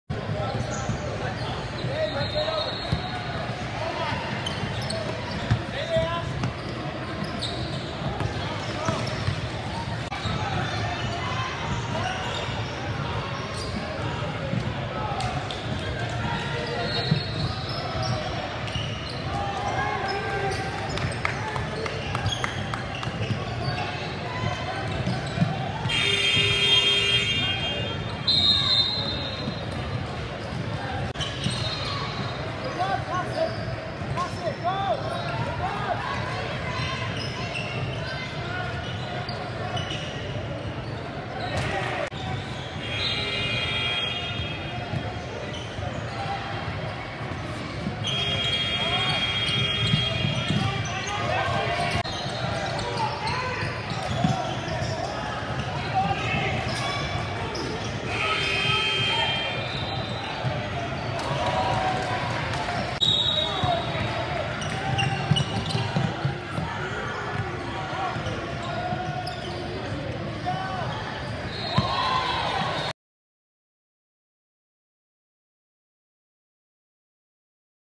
KBC gets the victory over San Diego Matrix, 67-30 - Grassroot 365 Invitational: West
Grassroot 365 Invitational: West.